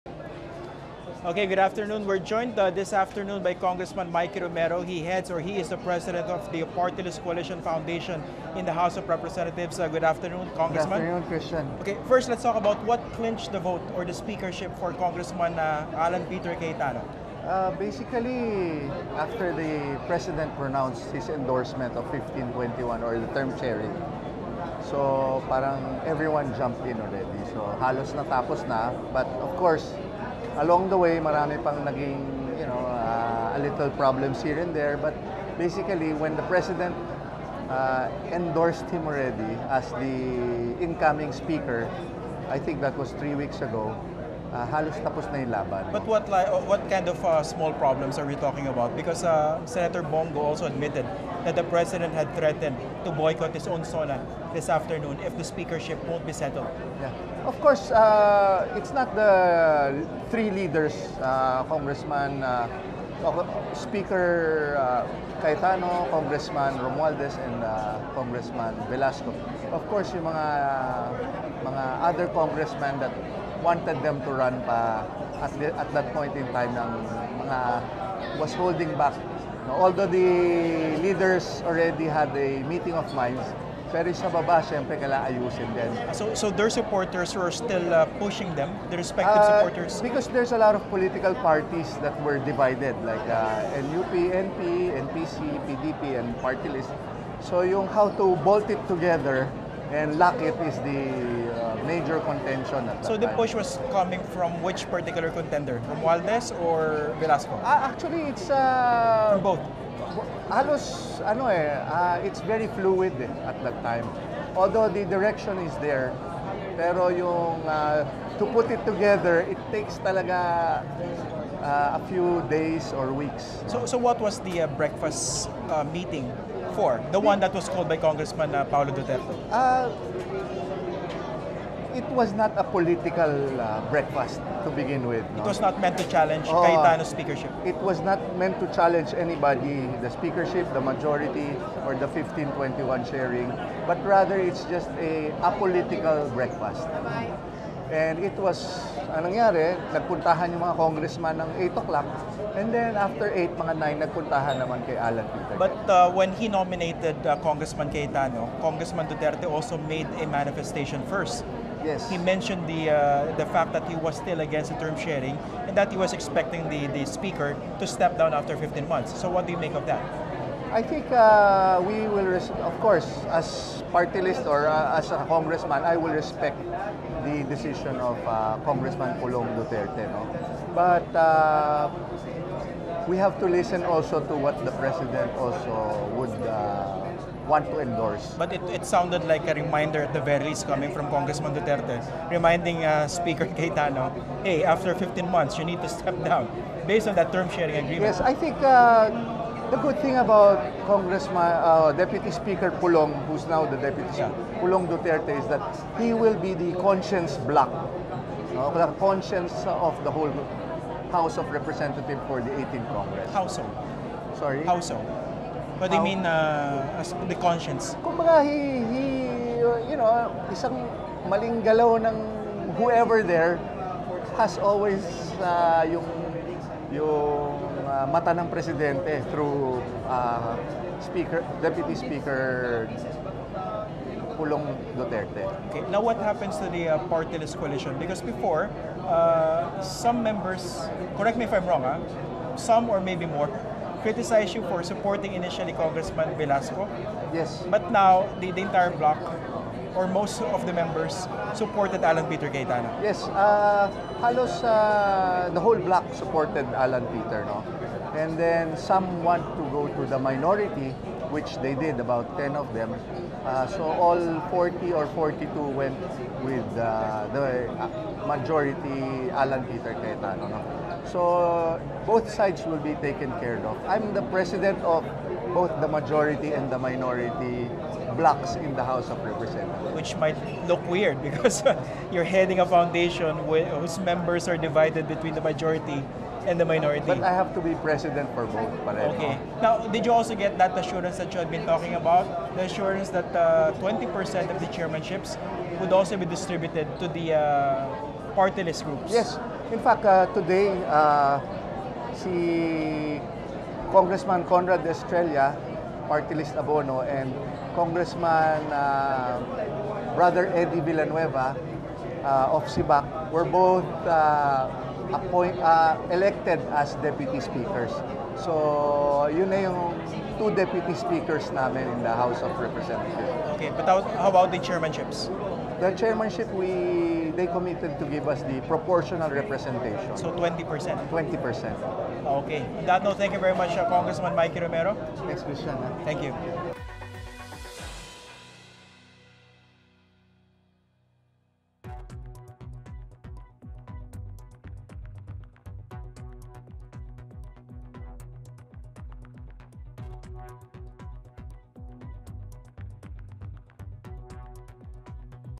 Romero on what secured House Speaker Cayetano's win amid coup rumors | ANC Highlights
Party-list coalition President Rep. Mikee Romero bares what clinched the vote in favor of Rep. Alan Peter Cayetano as House Speaker. This, amid persistent coup rumors despite President Rodrigo Duterte's endorsement.